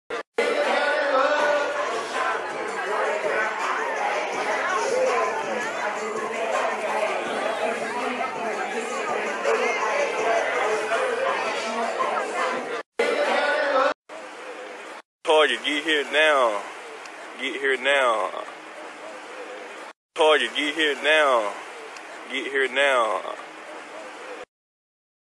Social Distancing, Mississippi Style
This was a "F the Covid-19 Party" held in Madison County, MS Saturday night.